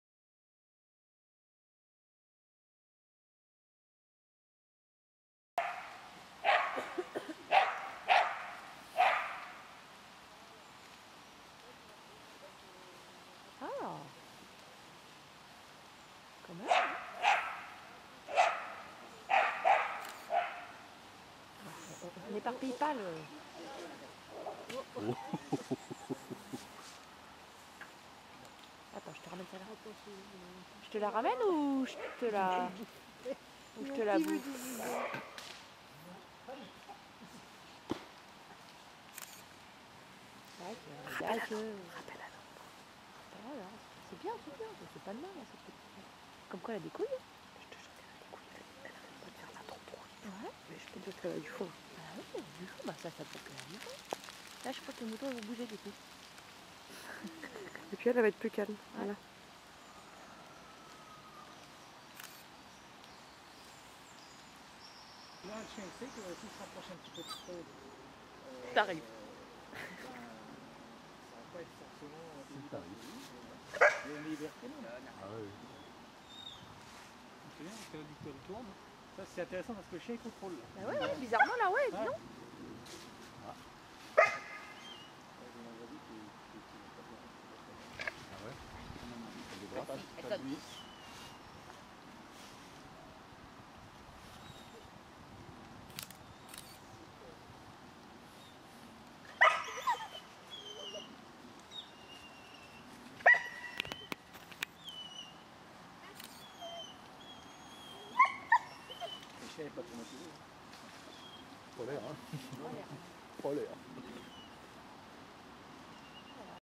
Gipsy - Test troupeau
04.11.12: test troupeau pour Doongara Gipsy, Kelpie femelle d'un an, à l'Alliance Bergère -Giencourt.